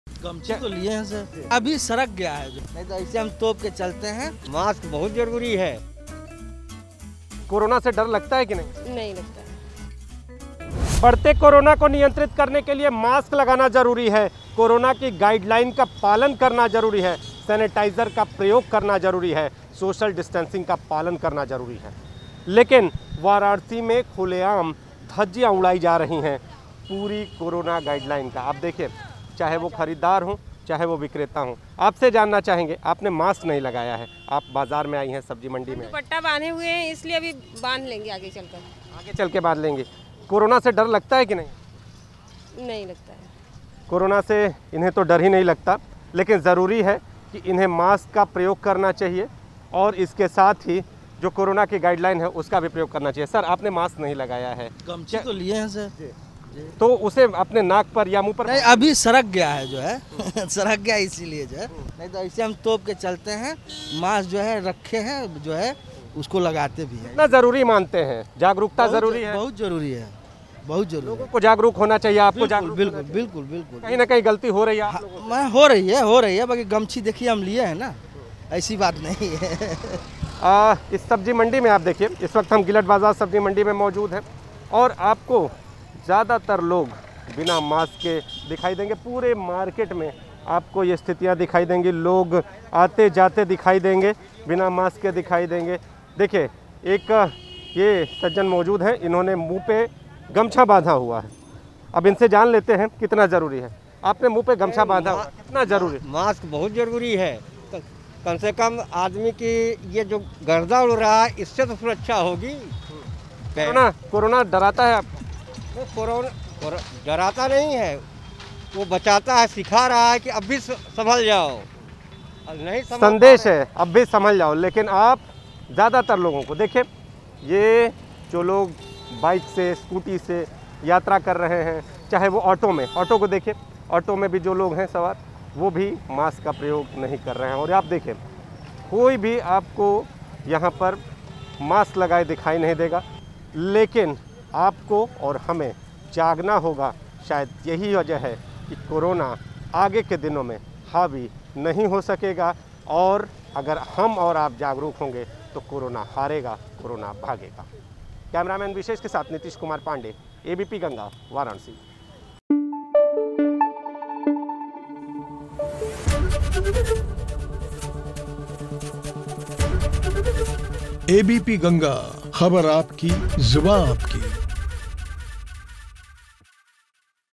कोरोना के डर पर क्या बोले काशीवासी ? | Varanasi | Covid 2nd Wave
वाराणसी में कोरोना को लेकर लापरवाही जारी है. लोग बिना मास्क के चहलकदमी कर रहे हैं. ऐसे में संक्रमण बढ़ने का खतरा है. आपको बता दें कि वाराणसी जिलाधिकारी ने चेतावनी दी है कि कोरोना गाइडलाइन का सख्ती से पालन करें, नहीं तो प्रशासन सख्ती बरतेगा, लेकिन गिलट बाजार सब्जी मंडी में लोग बिना मास्क के दिखे.